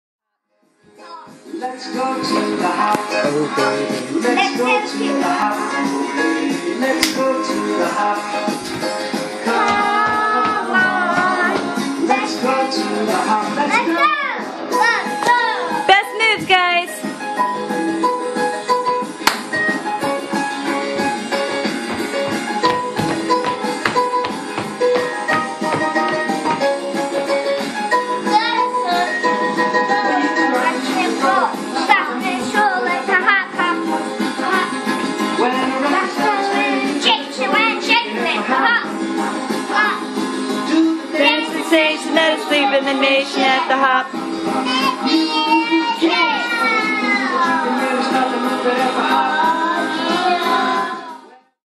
Kids dance in the kitchen
My kids and I have a dance party in our kitchen. I rest on Fridays, but that doesn't mean I don't move around. In fact, Friday evenings the kids and I put on some music and dance while we make dinner!